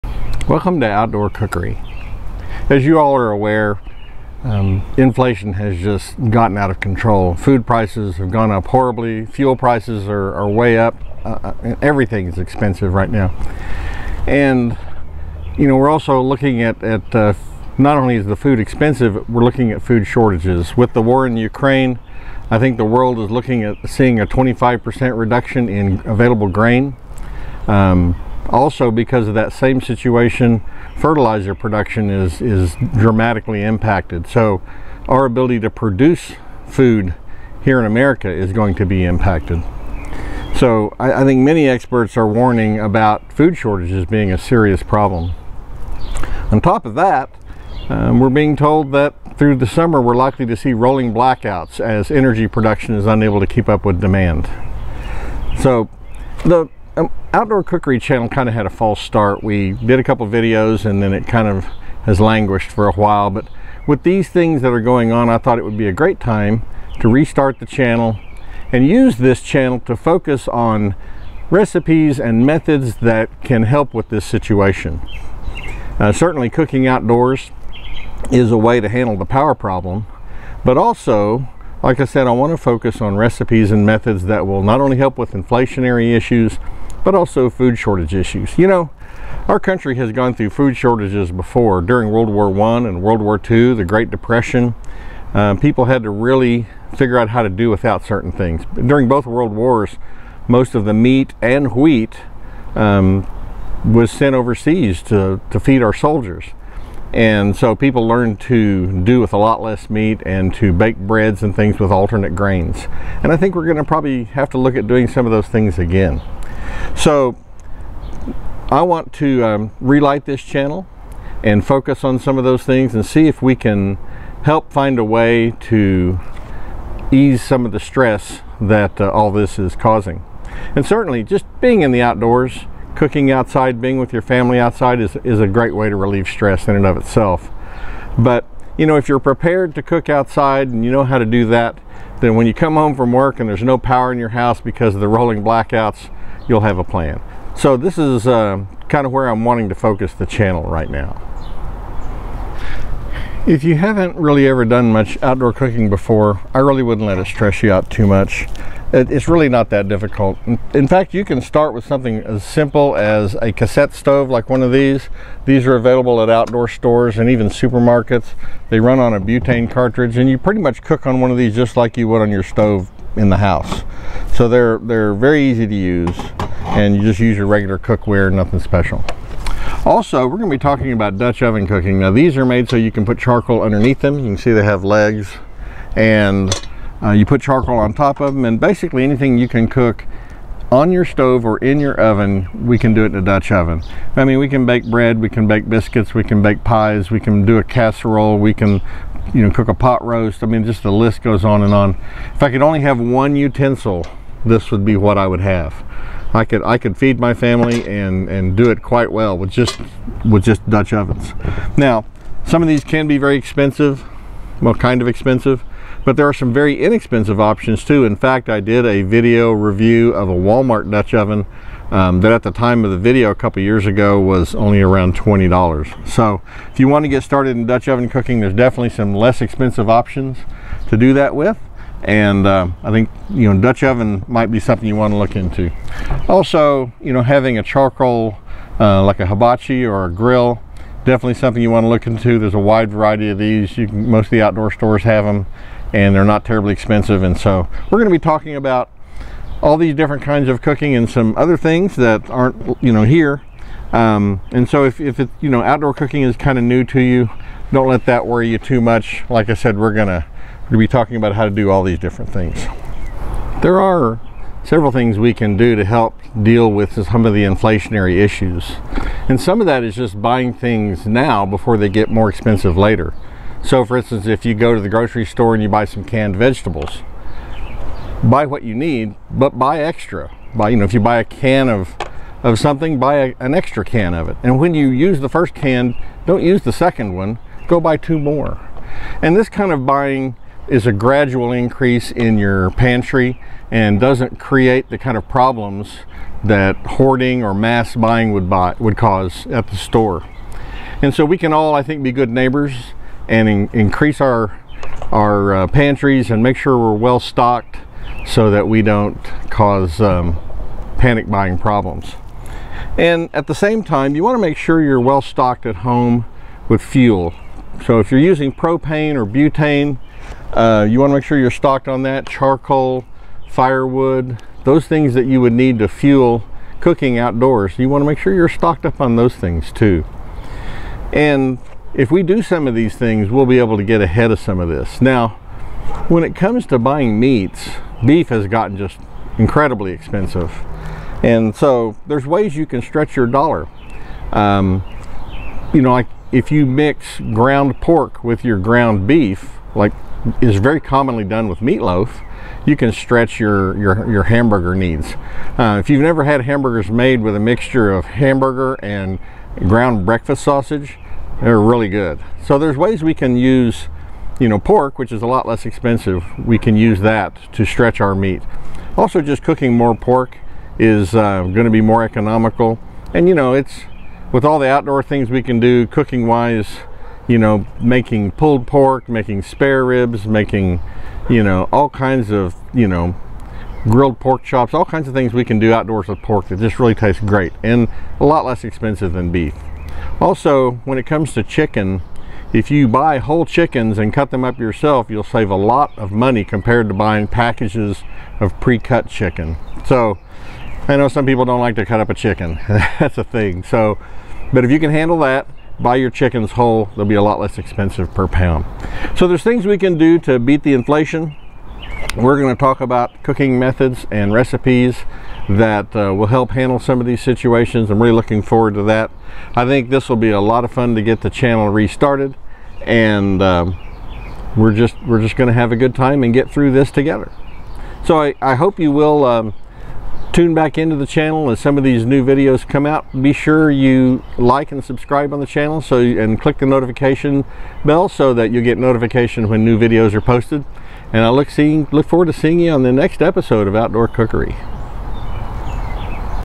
Fight Inflation and Power Outages with Outdoor Cooking
We are relaunching Outdoor Cookery!  Outdoor cooking is a great way to handle power outages, inflation and the stress of these difficult times.